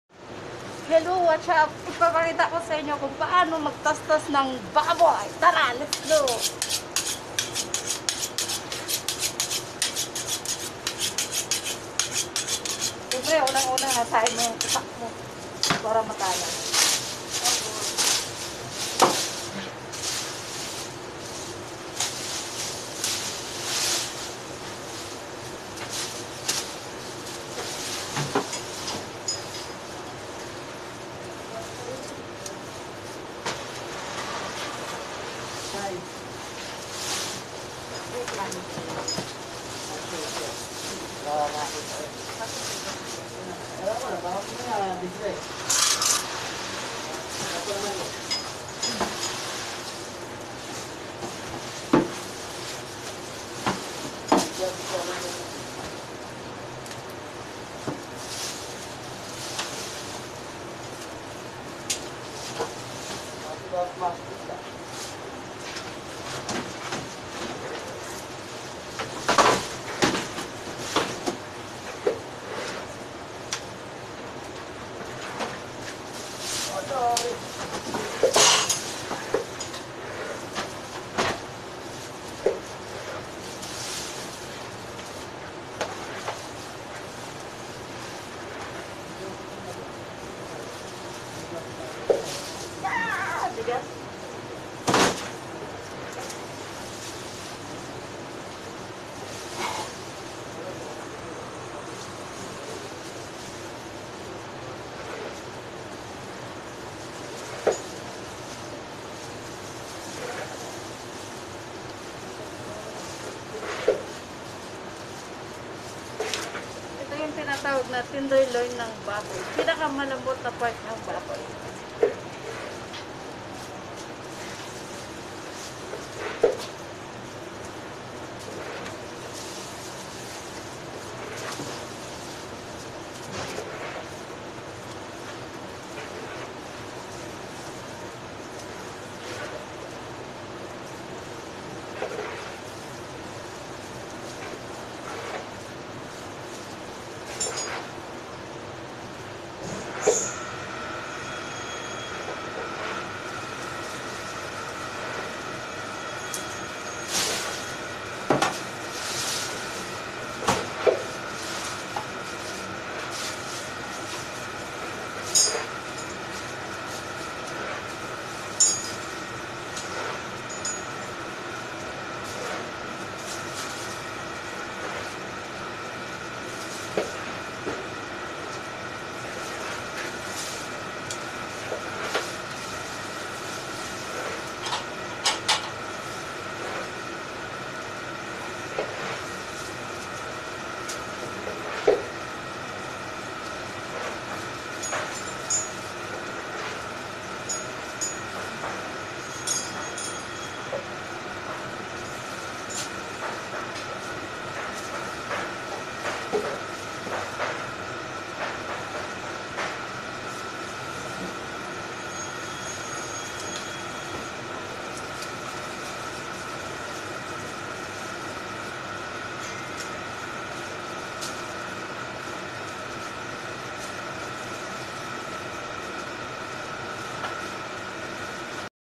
HOW TO DE BONE PORK#BUTCHERS TRAINING#WHAT PART OF PORK VERY TENDER#HOW TO CUT BIG PORK OR PIG
Schooling of butchers. All ready to apply in other country...cutter pork and beef..already masegs for training or how know by parts.